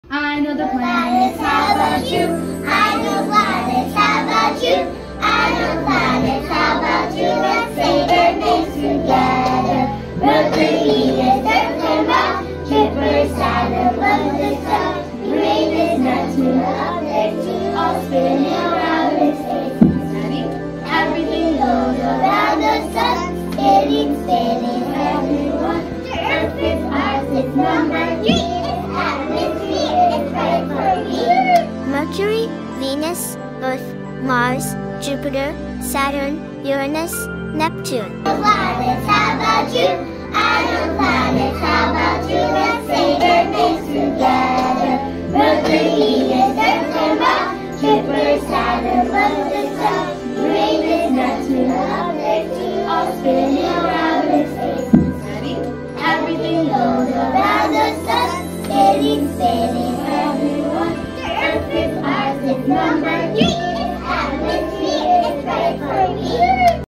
AR in TK Classroom
Our TK students enhance their solar system learning with Augmented Reality